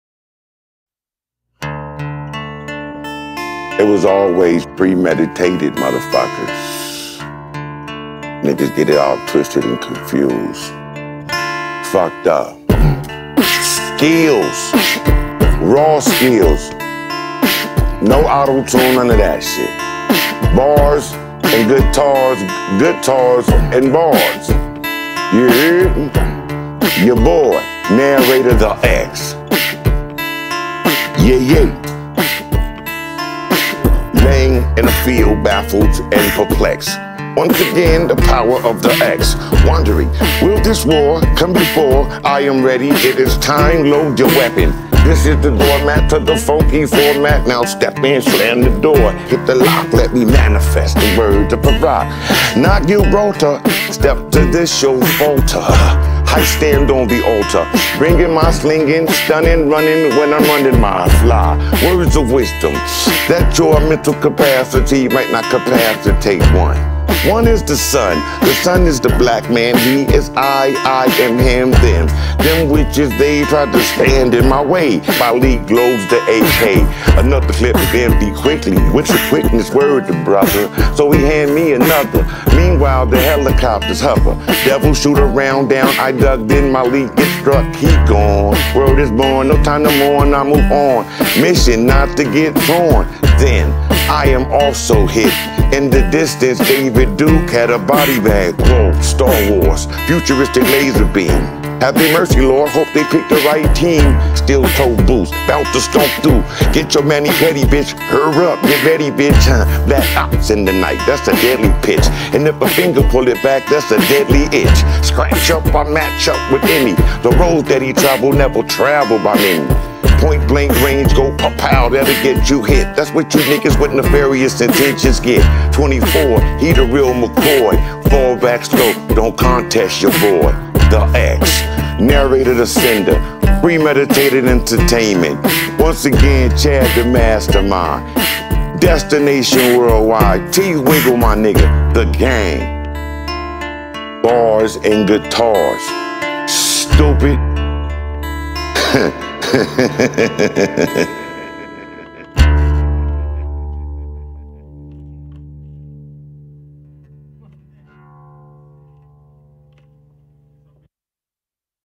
RBX x The Field x B&Gmix (LEGEND FILES)
The Legendary RBX comes correct with the BARS over the acoustic beatbox in true B&G style like you knew he would!  This is a Bars And Guitars bucket list episode, and such an honor to shine the light on this 1 of a kind Legendary Artist who inspired so many, and is consistently releasing new music even today! Tap in with RBX and stay up to date with all of his releases, shows and merchandise as he continues to keep the the West Coast active and thriving🔥🔥🔥🔥🔥